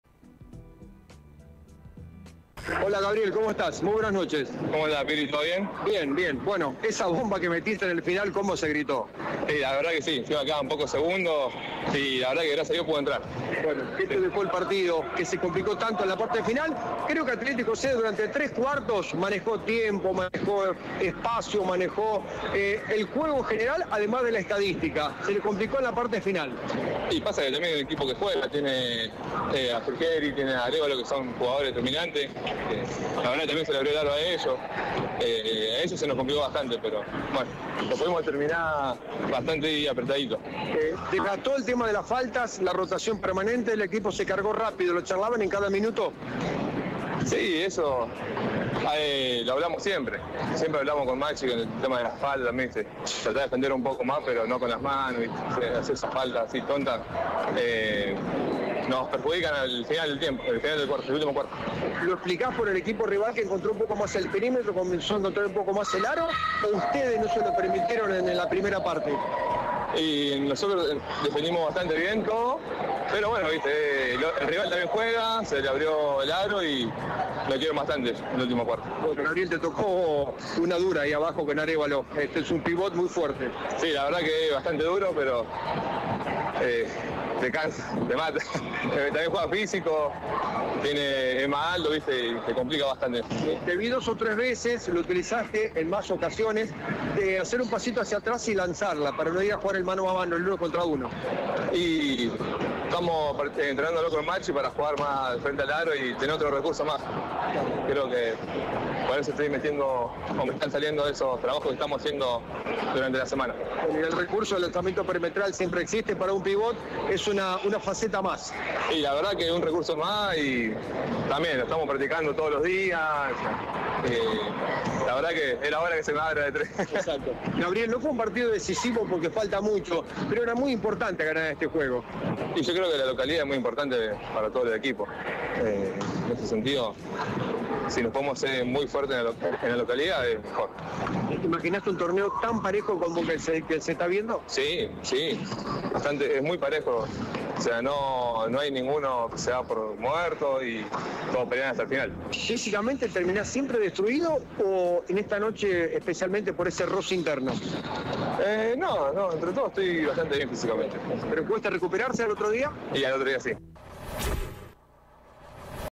Torneo Pre Federal Gabriel Romano Atletico Ceres le ganó 91 86 a Platense El pivote anotó 2
Torneo Pre Federal. Gabriel Romano -   Atletico Ceres le ganó  91-86 a Platense -  El pivote anotó 24 puntos